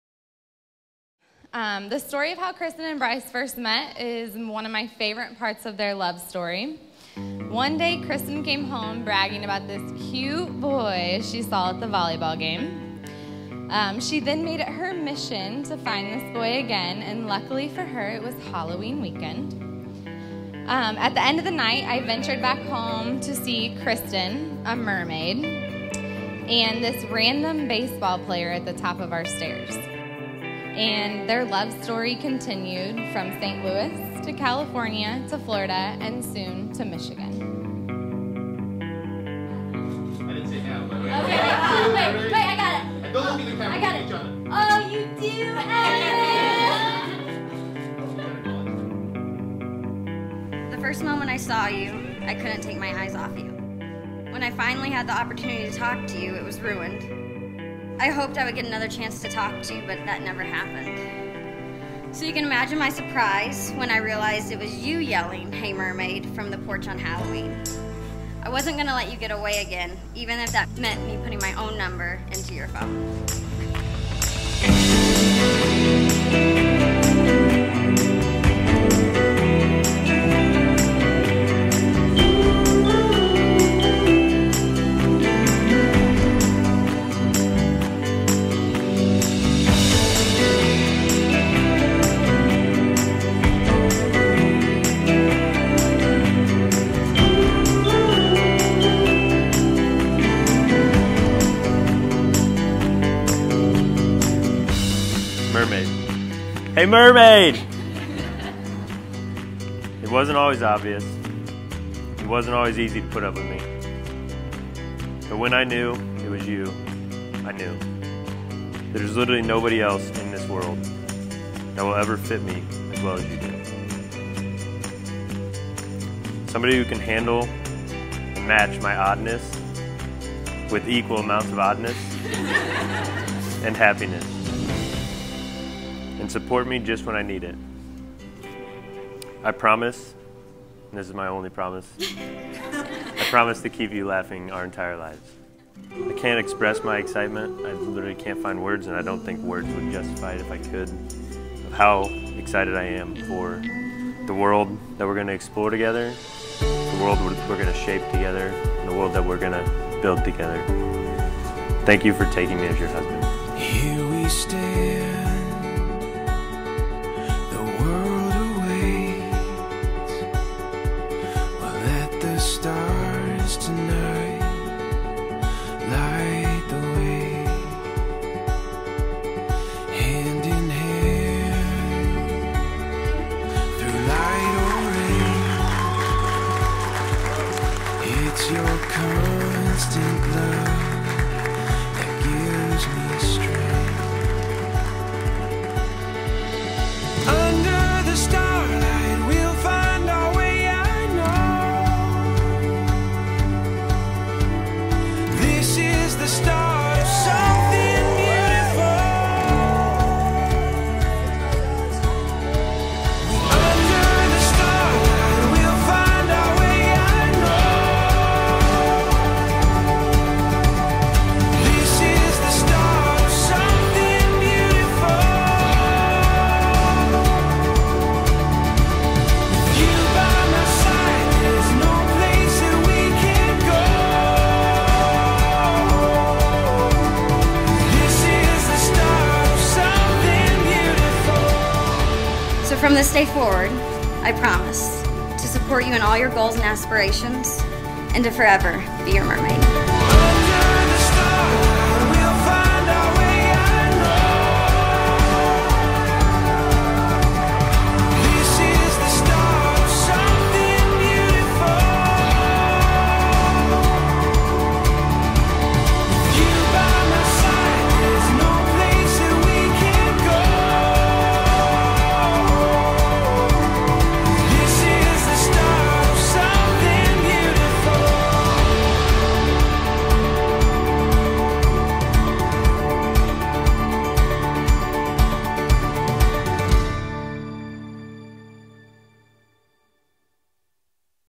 "Hey Mermaid!" Kristen + Bryce's Wedding Highlight
Kristen & Bryce’s wonderful unique love story captured the hearts of all their family and friends. Not only did they both reference their first meeting in their vows but the matron of honor and best man did as well. The Conservatory made for a charming back drop for their nuptials, where the urban ambience of Neo on Locust in the heart of downtown St. Louis offered a fun and festive atmosphere. During the reception the beautiful bride surprised her new husband with a special celebrity guest, Fred Bird who made the party that much more fun!